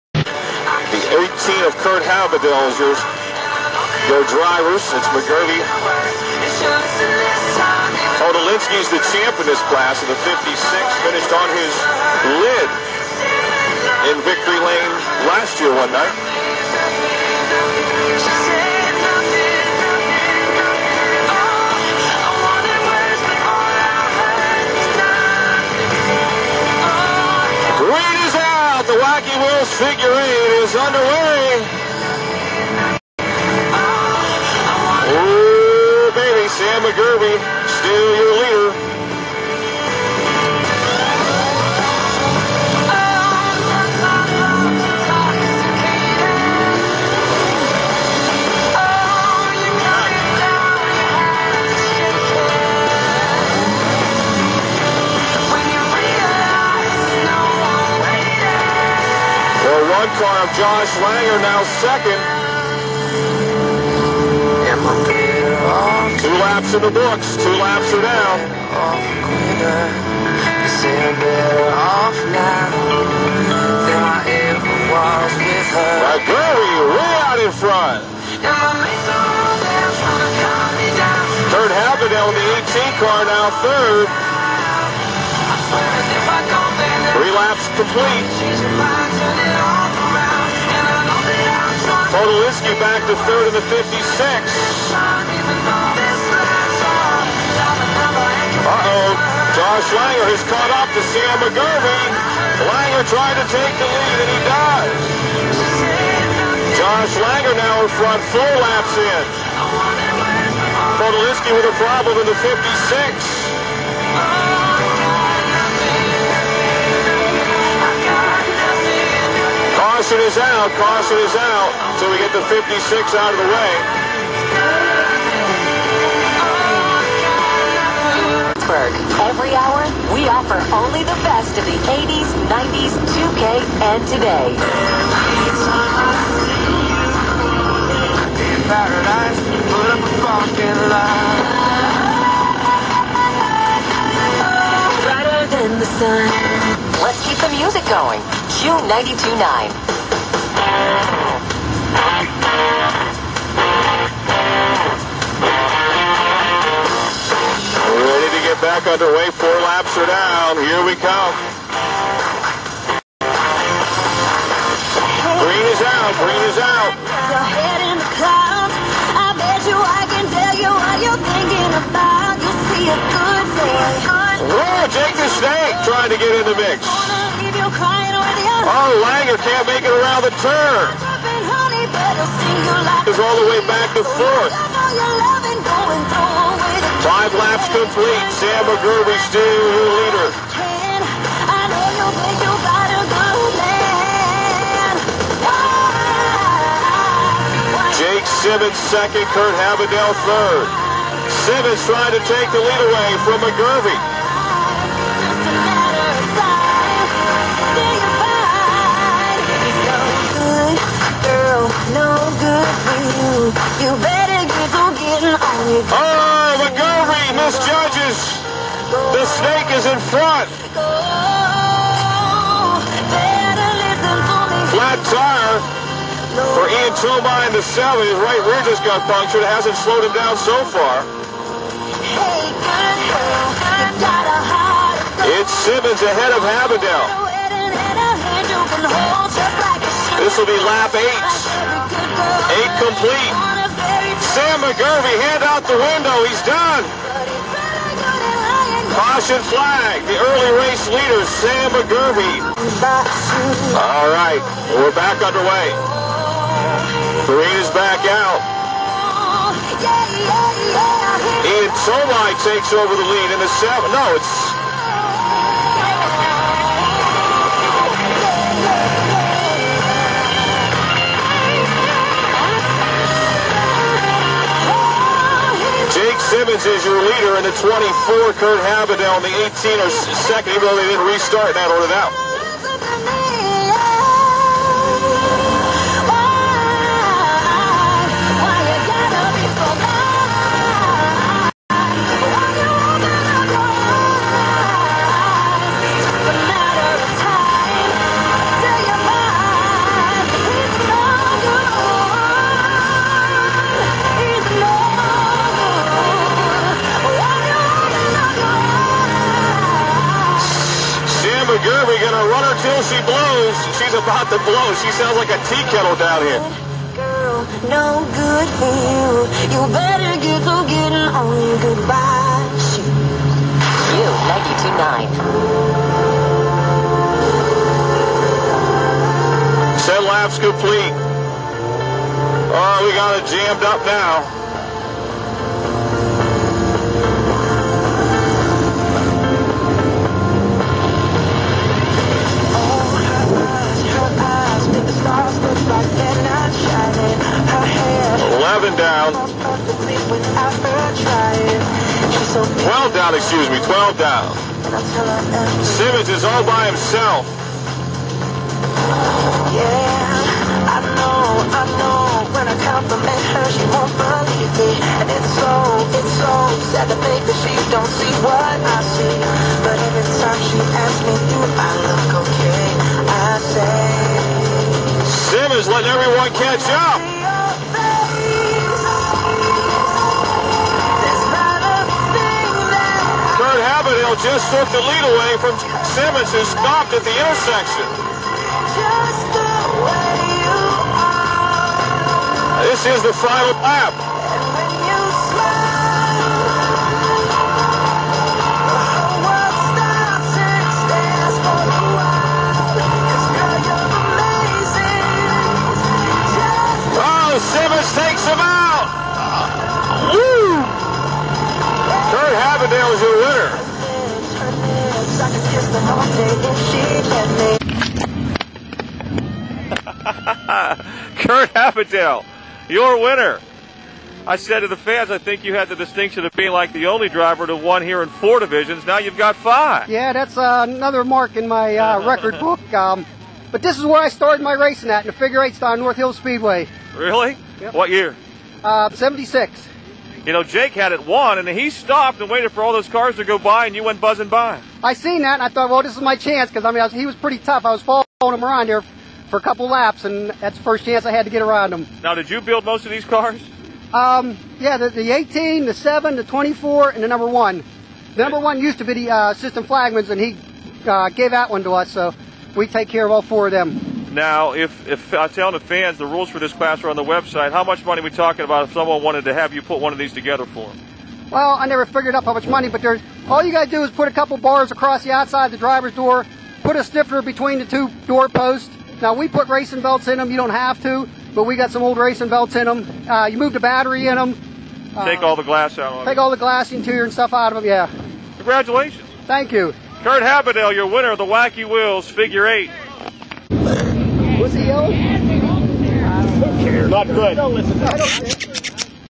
wacky wheels race 5-13-12.wmv
Wacky wheels race at Pittsburgh pa motor speedway may 12,2012
and interview with winner Kurt Halbedl